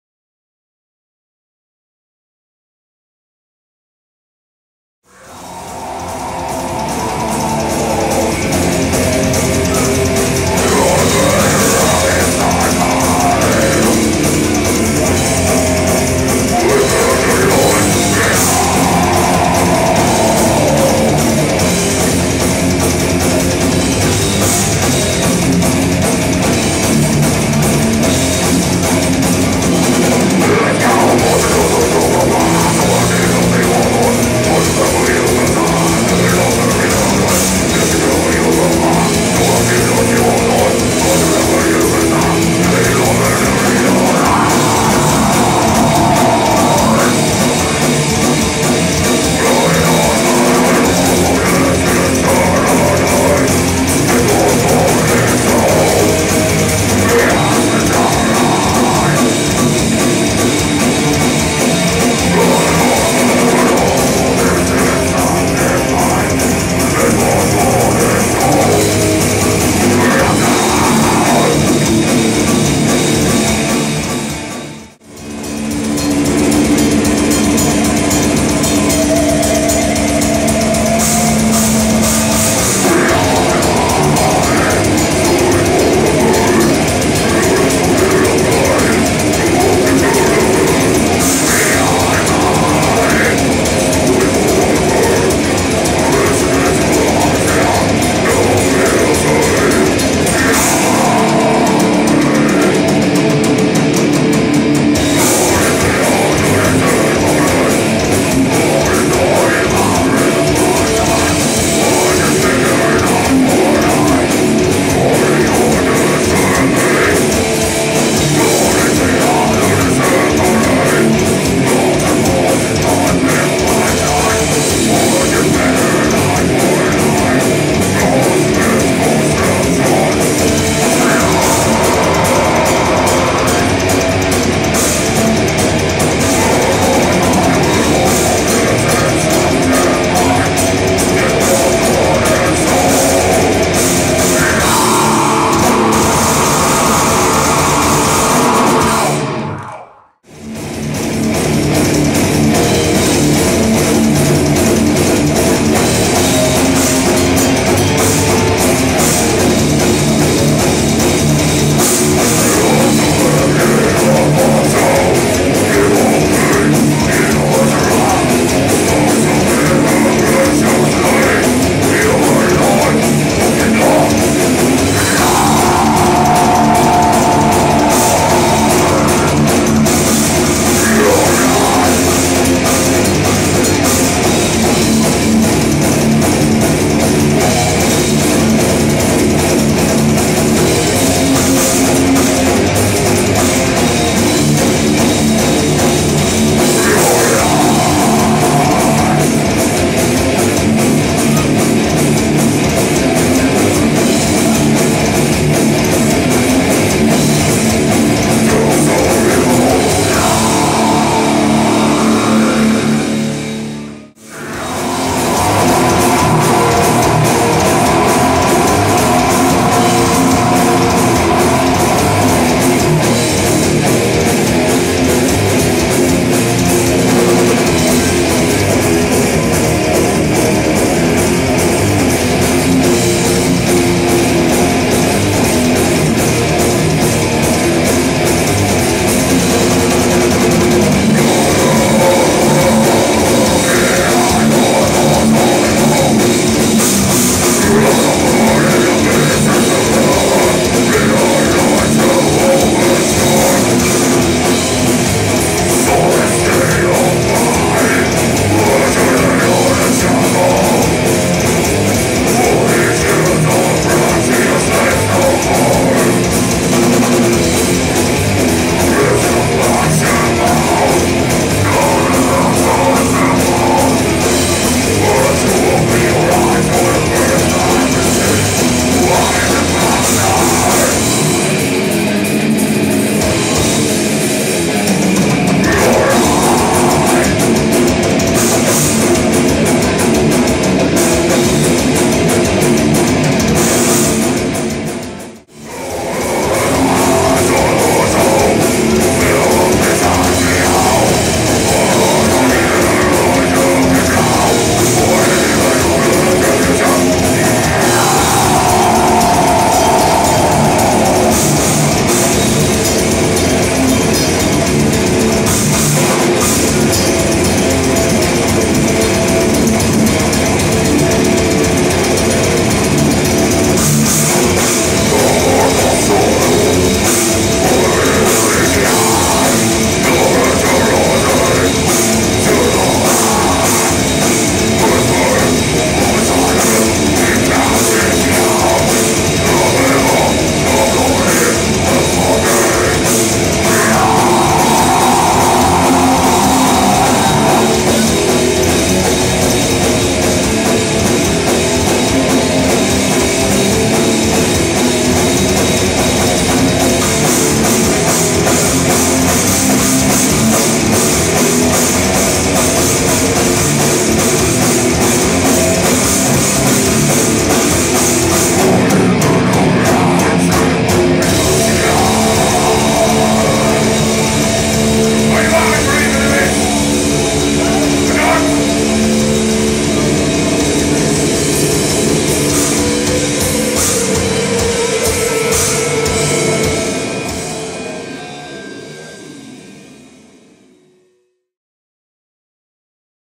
Graven Image - Dutch Death Metal Footage
Graven Image is a Death Metal band from Holland. This was recorded at O.J.C. Maddogs Groesbeek on 15th of January 2010, the Netherlands.

Graven Image is:

Royy
Tom
Dave
Arno
Waltr
Ask